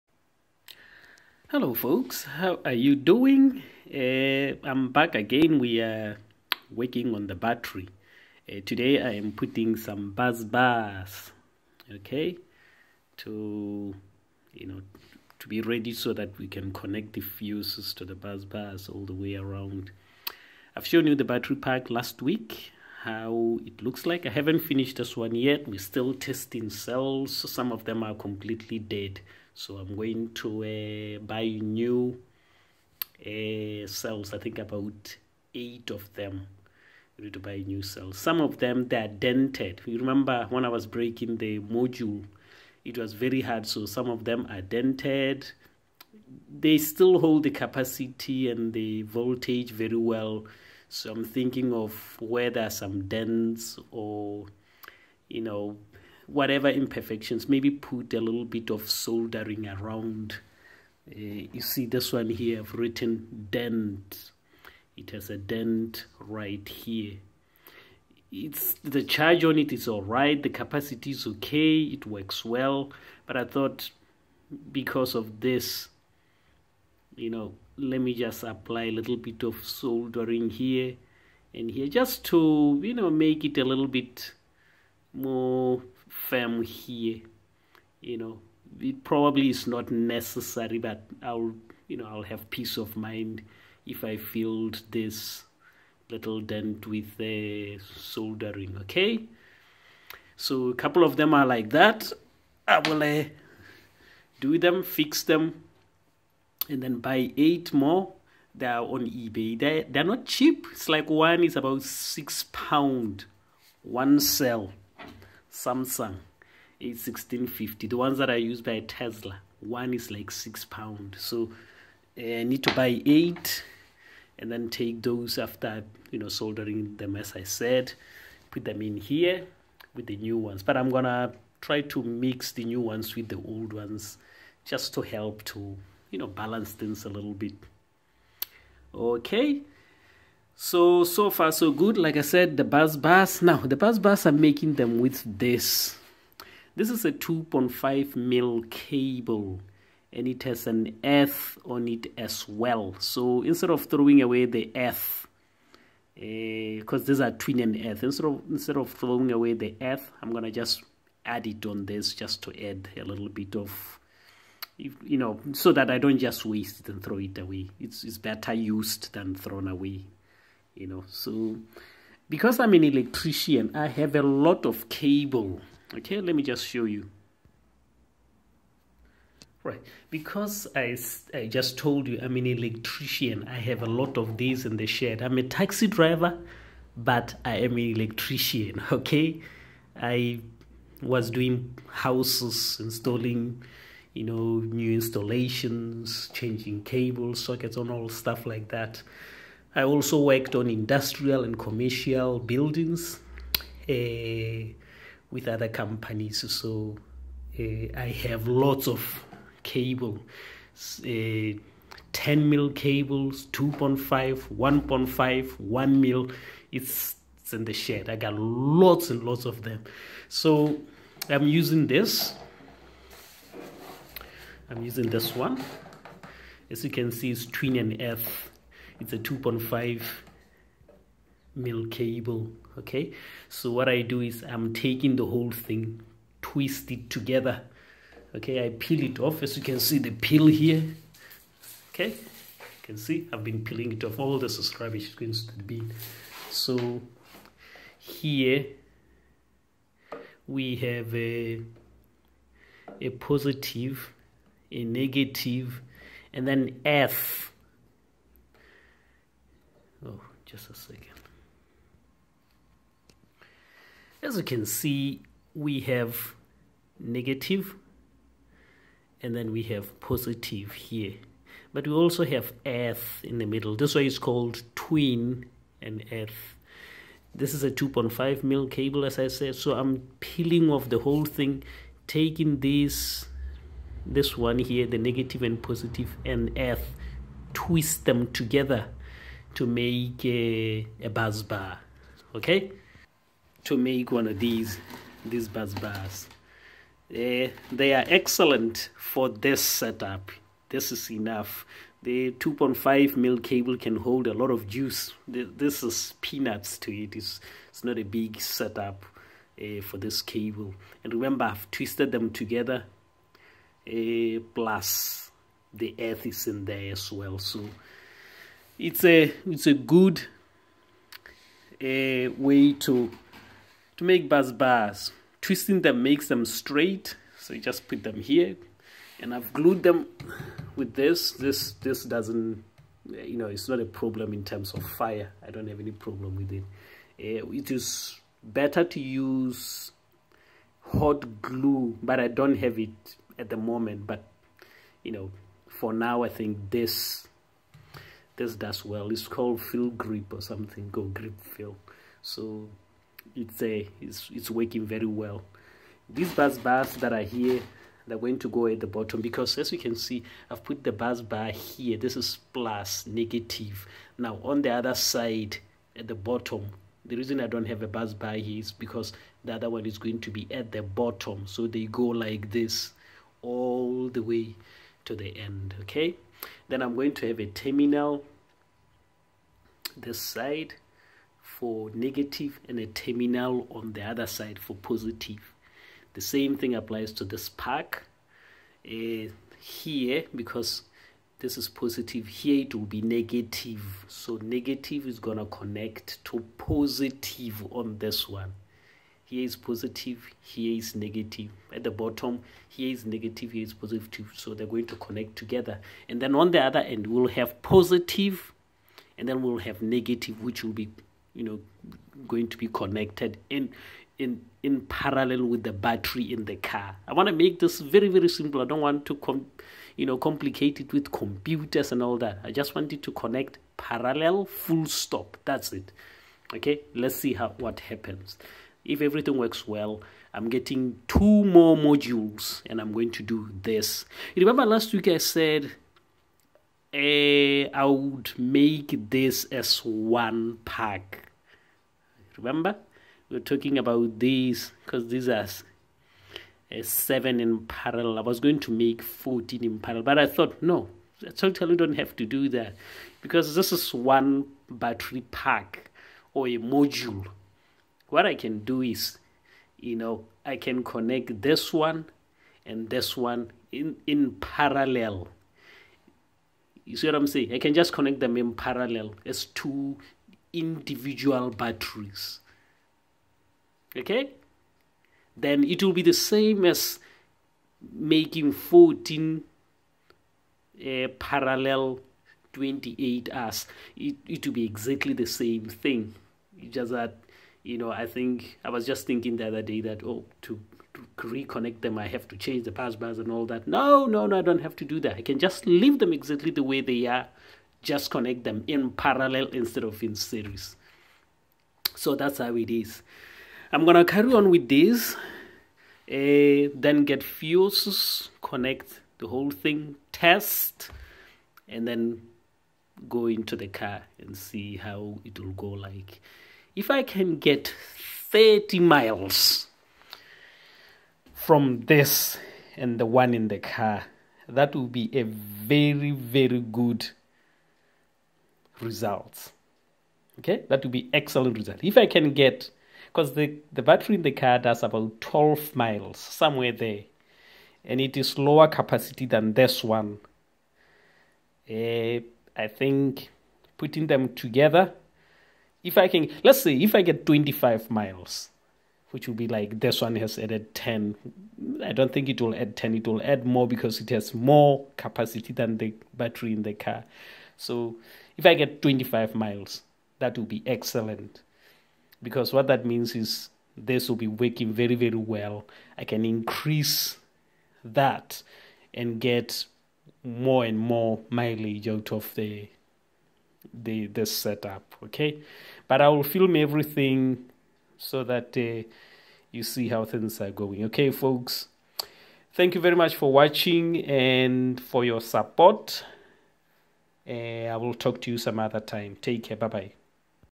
Tesla Battery Pack - Making Busbars
Hi guys, back again. Today I'm just showing and explaining how my battery pack is going.  Made busbars from 2.5mm cable with its earth(cpc) cable. Everything is going excellent. As always, if you notice error, you are more than welcome to query. Thank you guys.